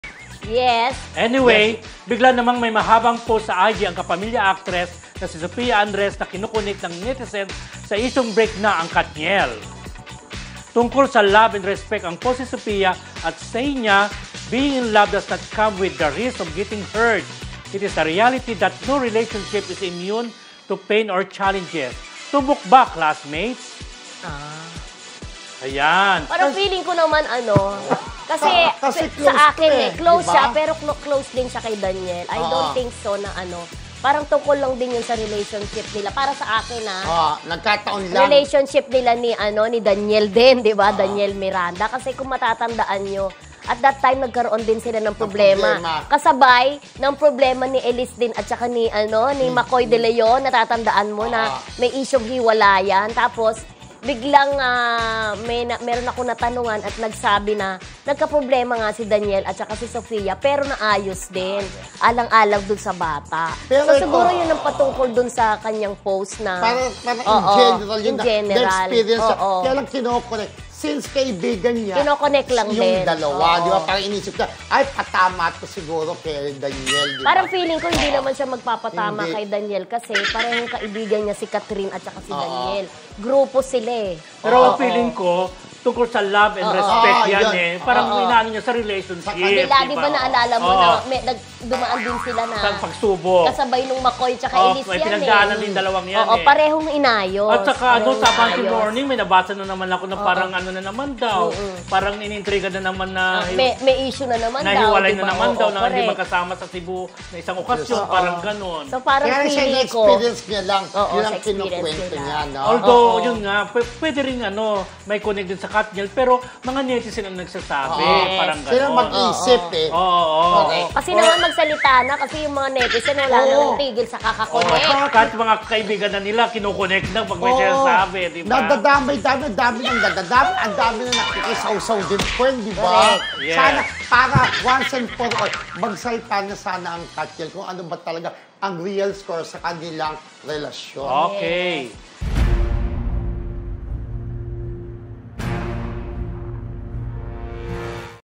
Sofia Andres post kaugnay ba sa KathNiel breakup? | MARISOL ACADEMY QUICKIE
Nag-post kamakailan ang aktres na si Sofia Andres sa kaniyang Instagram account patungkol sa love and respect na diumano’y related sa issue ng KathNiel.